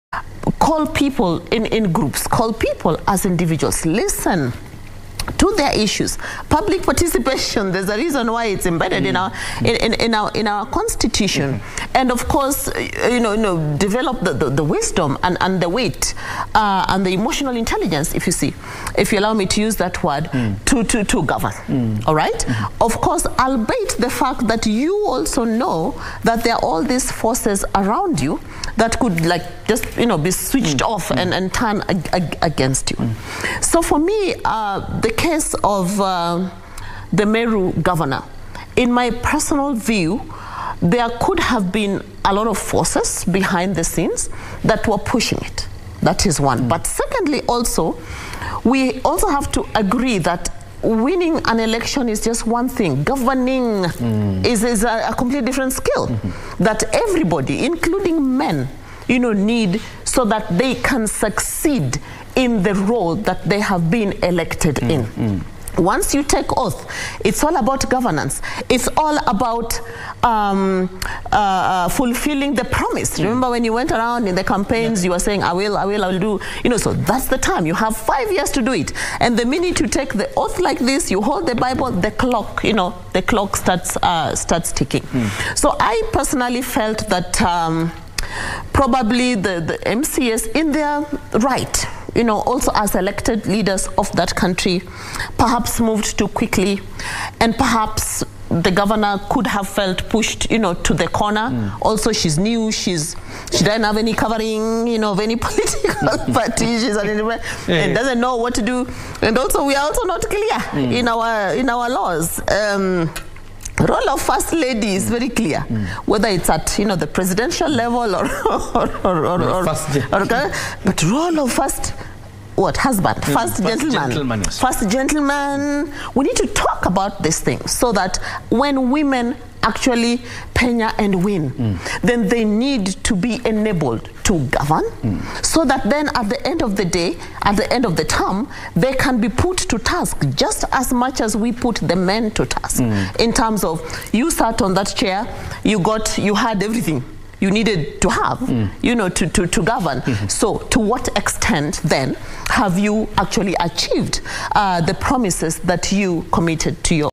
Betty : The role of the first gentleman needs to be clearly defined in our constitution
Women in Leadership :

Betty : As a leader, call people and listen to their issues. There could have been external forces pushing Governor Kawira's issue, but the role of the first gentleman needs to be clearly defined.